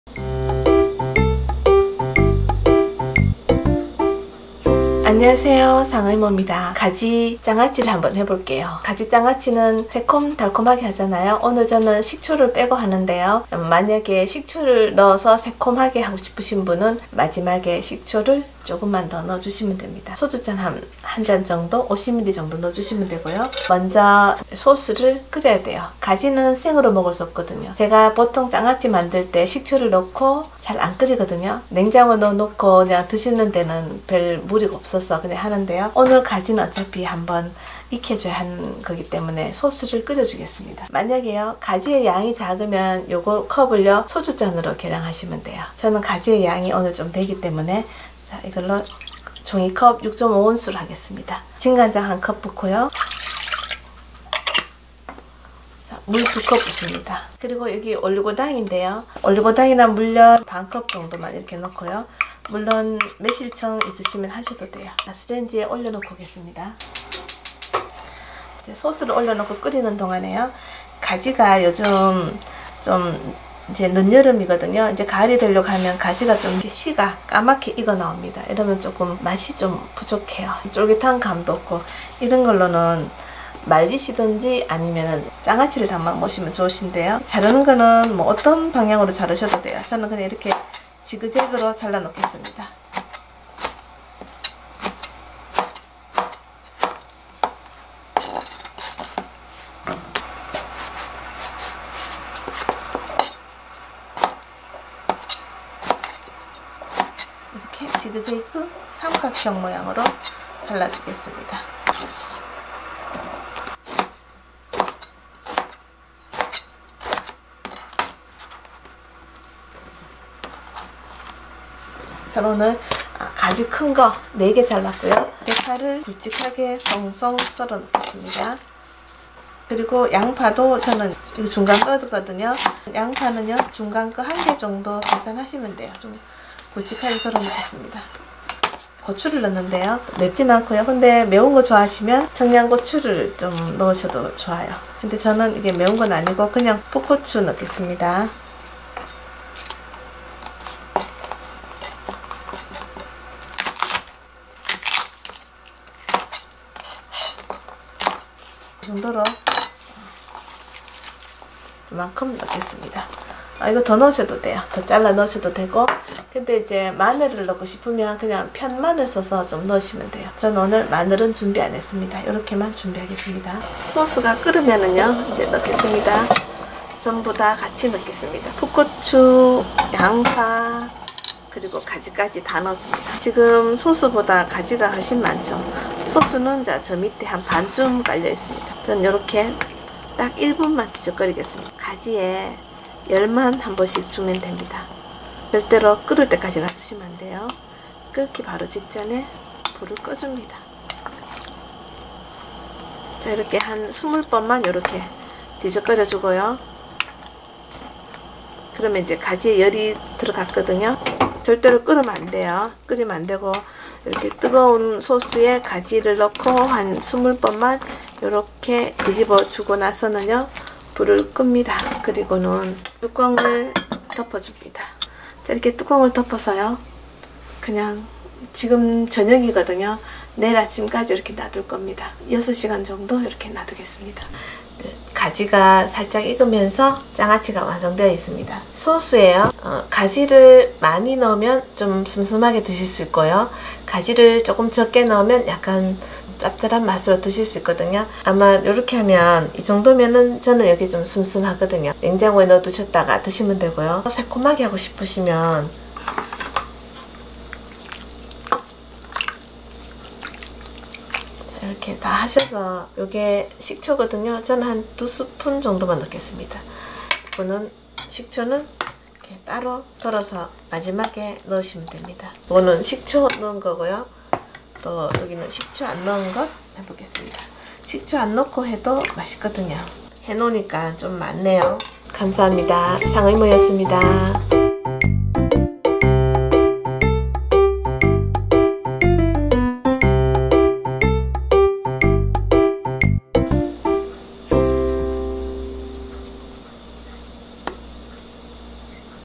쓰리고로갑니다!! 쉽고 ,맛있고,싸고,3고입니다/젓가락을 멈춤지못하게하는맛 [상어이모.SANGEOIMO]
안녕하세요?가지로 만드는 가지장아찌입니다 !!
몸에좋은 가지 많이 드시고 건강하십시요!!~~

가지장아찌 소스
가지150g4개/양파1개/대파/풋고추
진간장1컵/물2컵/올리고당이나 물엿반컵/식초는 취향대로 1소주잔(50ml)혹은 나누어담아서 2스푼